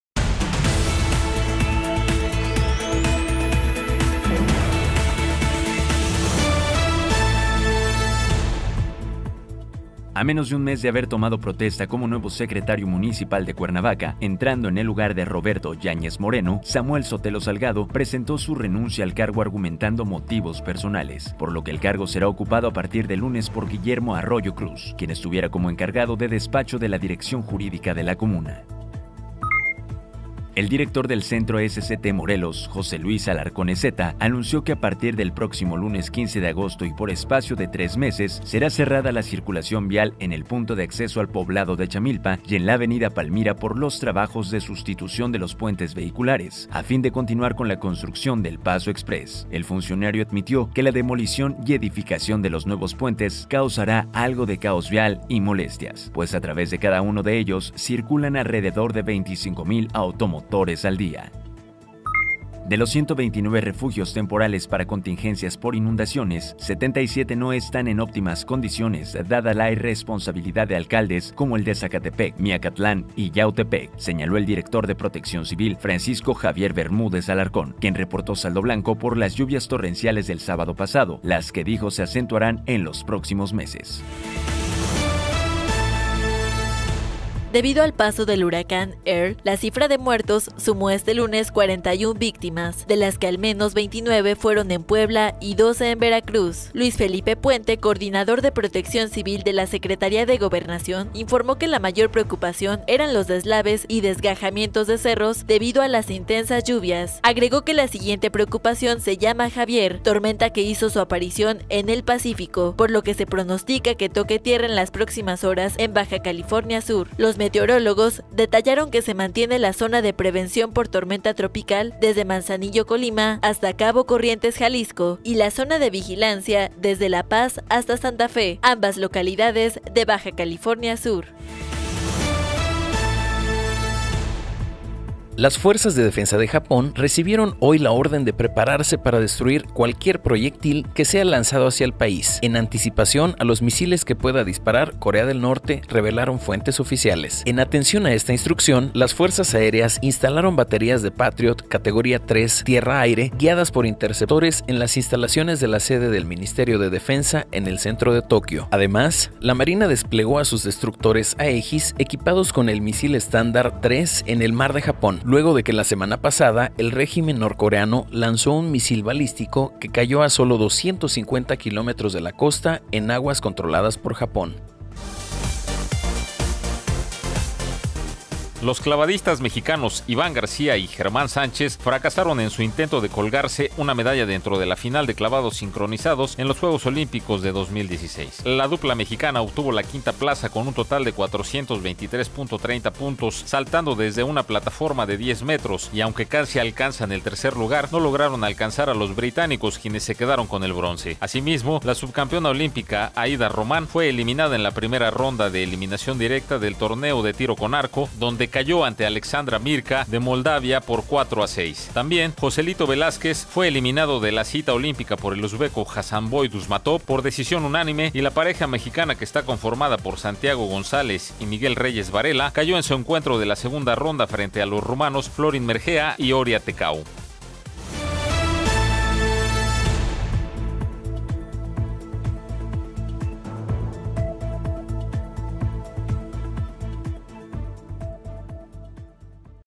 Informativo Morelos Habla Tv 08-Ago-2016
Renuncia secretario municipal de Cuernavaca. Ésta y más noticias en MorelosHablaTv